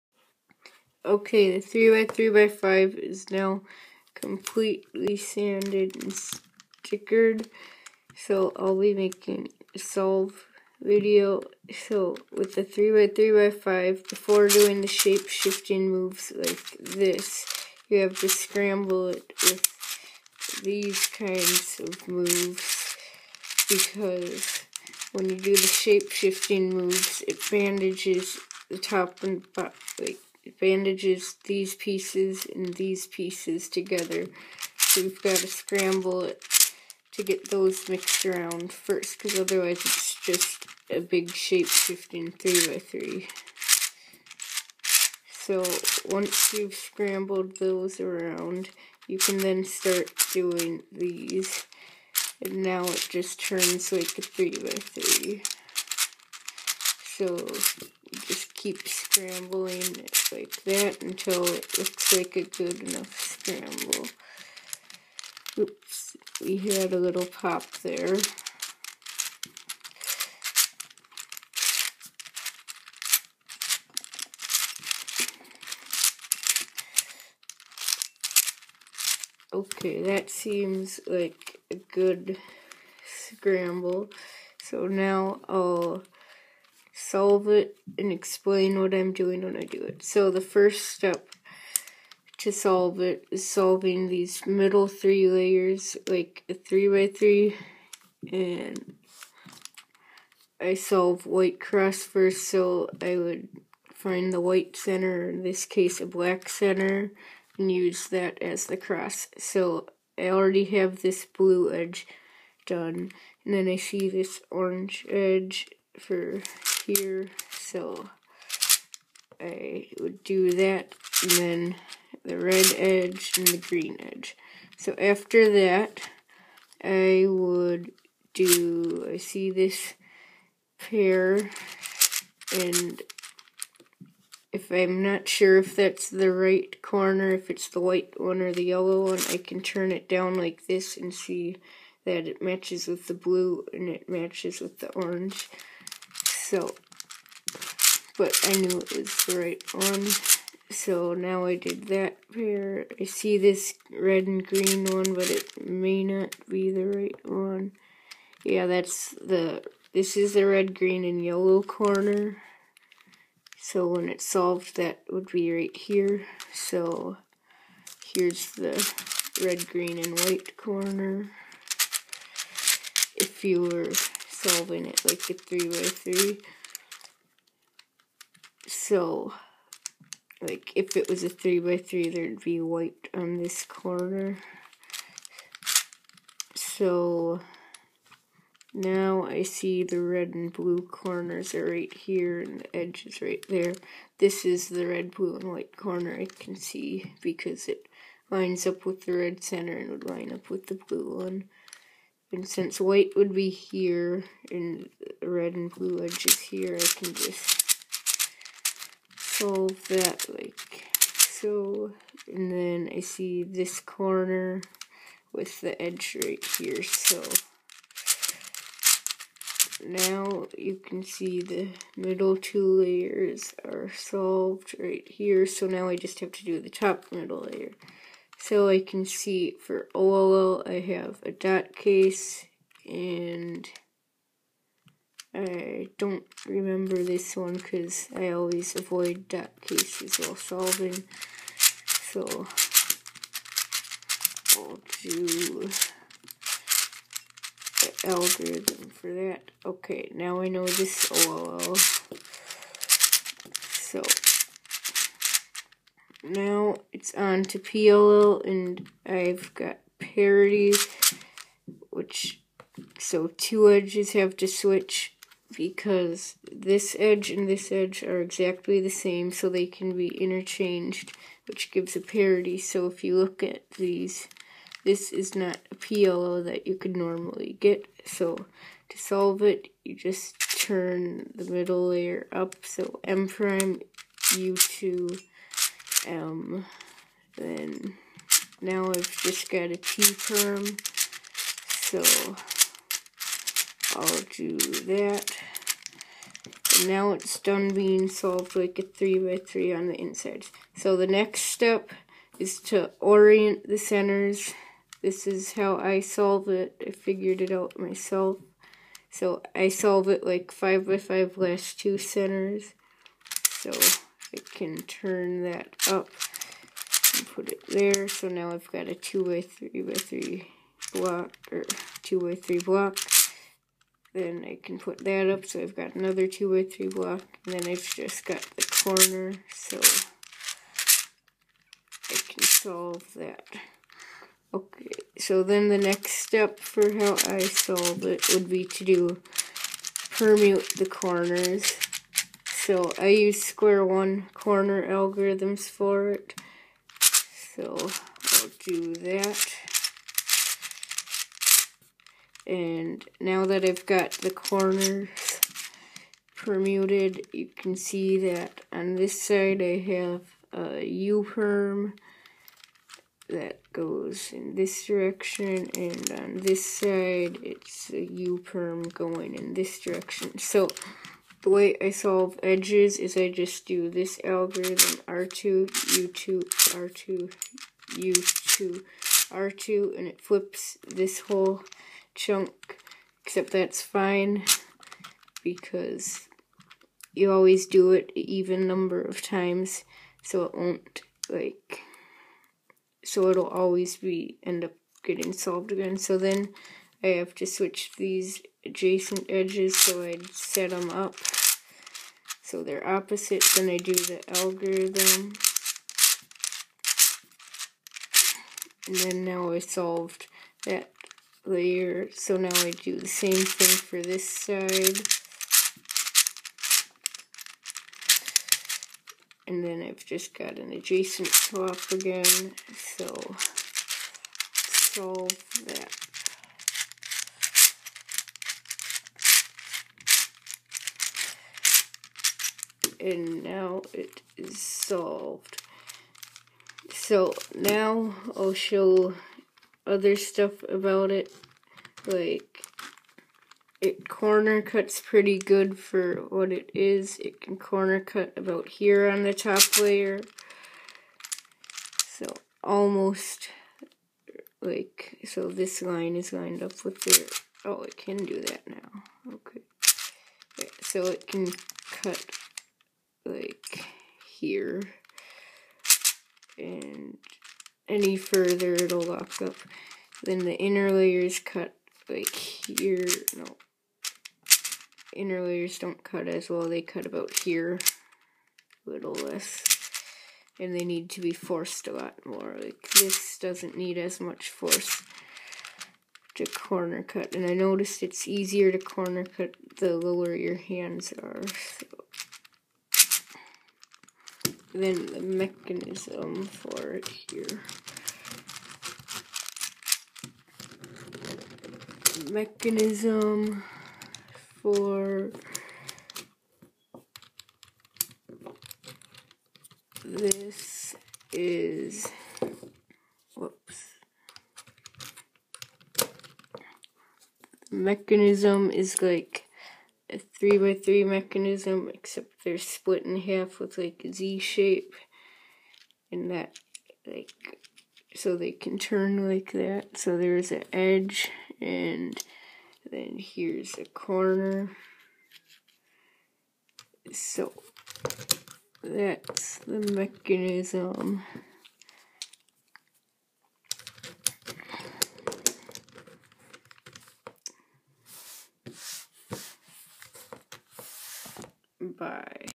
3D printed half proportional 3x3x5
This shows the solve and function of my 3d printed 1/2 proportional 3x3x5. This is all my own design, and all pieces have been hand sanded for better performance.

The last minute of the video shows some of the internal mechanism and corner cutting abilities.

Printed on a Lulzbot Mini in PLA.
I also make my own stickers and cut them on a Roland SX-15